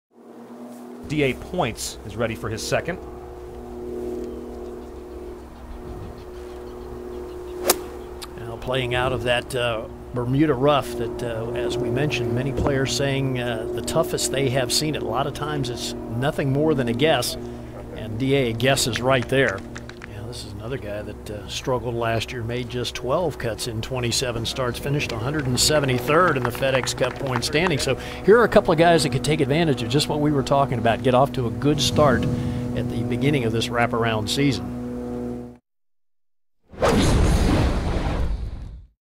D.A. Points' approach sets up short birdie at McGladrey
In the opening round of The McGladrey Classic 2014, D.A. Points hits his 95-yard second shot on the par-4 5th hole to 8 feet and makes the putt for birdie. .